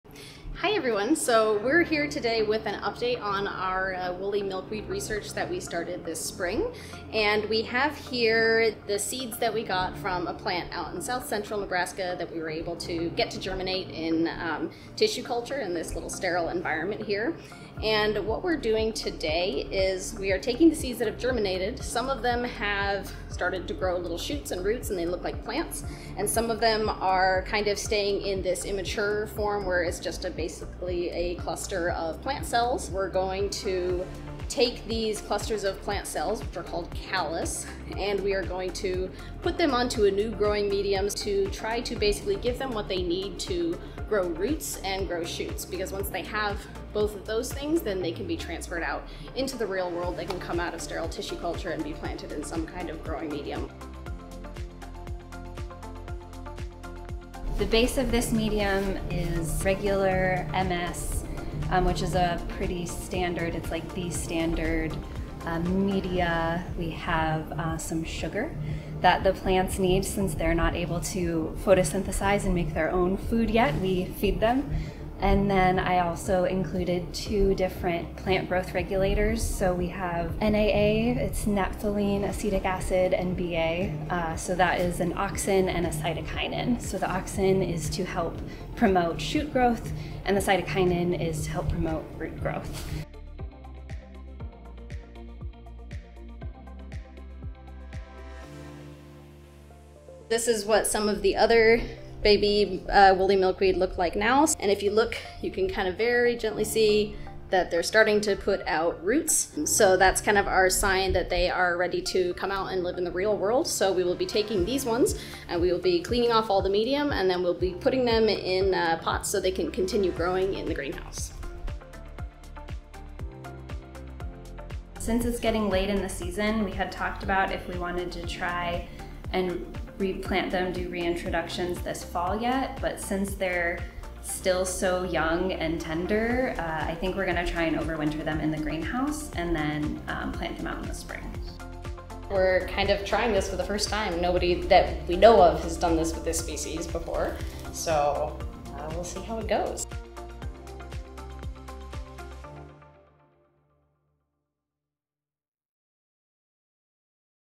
UPDATE! Woolly Milkweed Conservation
Woolly milkweed update! Since receiving 54 viable woolly milkweed (𝐴𝑠𝑐𝑙𝑒𝑝𝑖𝑎𝑠 𝑙𝑎𝑛𝑢𝑔𝑖𝑛𝑜𝑠𝑎) seeds earlier this year, our conservation team is beginning to see new growth. While some of the tissue cultures are still just clusters of plant cells called calluses, others have begun to develop several stems and roots, meaning that they are one step closer to being reintroduced back into their home prairie. Each of these developing plants plays a vital role in helping this Nebraska native species recover from its declining population. Take a look at the next step in the process as our conservation team transfers the woolly milkweed into new media to help each baby plant keep growing strong.